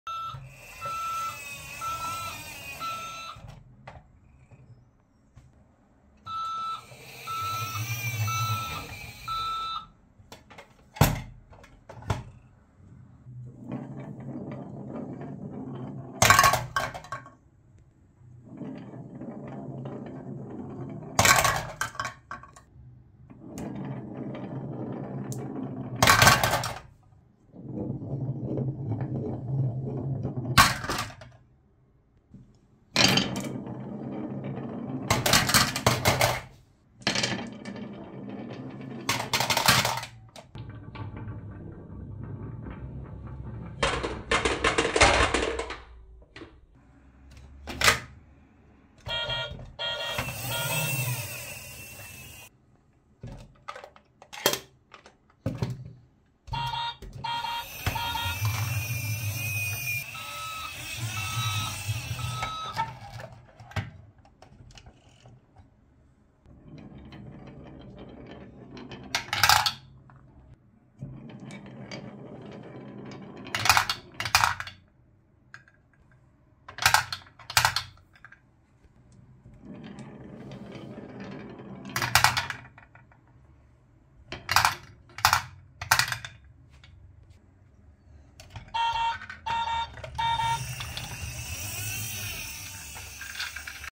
Marble Run Race ASMR ☆ HABA Slope, Dump Truck & Garbage Truck #10
Marble pops out vigorously. Marbles running from the HABA slope are stored in dump trucks and garbage trucks. Marble looks very beautiful when viewed in slow motion. Part 10.